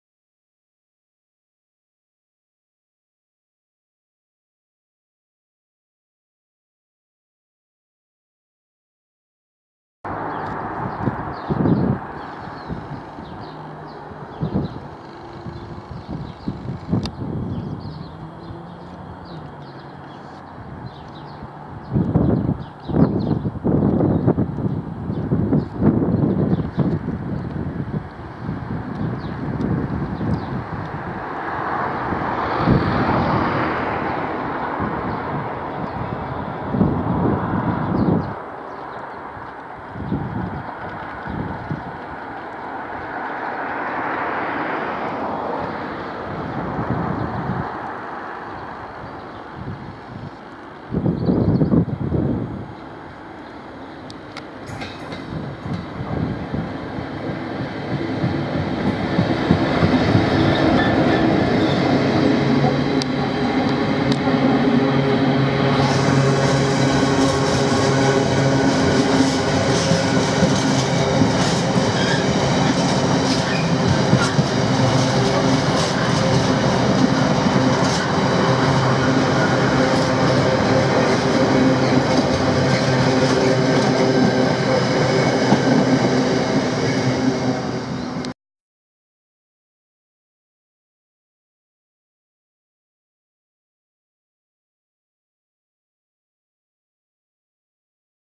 Freightliner Class 66 & 70 No's. 66512 + 70016 + 70003 on 6E53 @ Denton Jn on 29.03.20 - HD
Freightliner Class 66 & 70 No's. 66512 + 70016 + 70003 are seen here passing Denton Junction working the 6E53 Crewe Basford Hall - Hunslet Yard on the 29th March 2020.